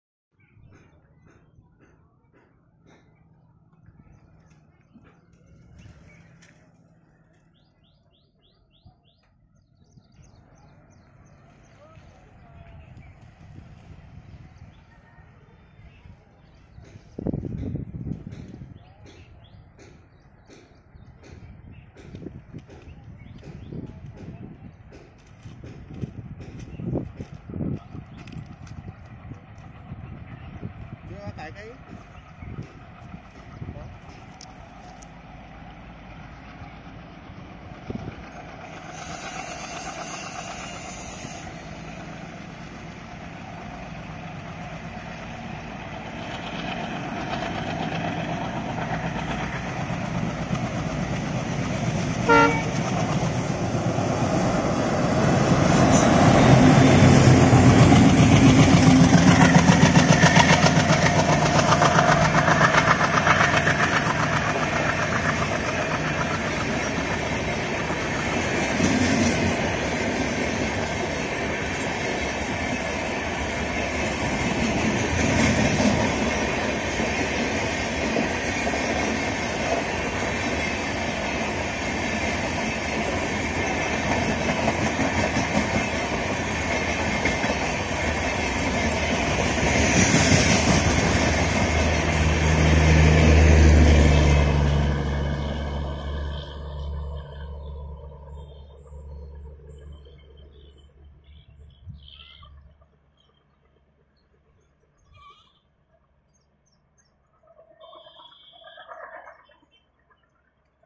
जिस पटरी से ट्रेन जा रही थी उसी पर चल रहे थे बेवकूफ लोग। 17623 नांदेड़ - श्री गंगानगर एक्सप्रेस।
17623 Hazur Sahib Nanded - Shri Ganganagar Express Via Ahmedabad accelerating after departing Akola Junction.

Locomotives :- 14577R + 14984 WDG3A Twins from Diesel Loco Shed Guntakal.

Location :- Dabki Road, Akola.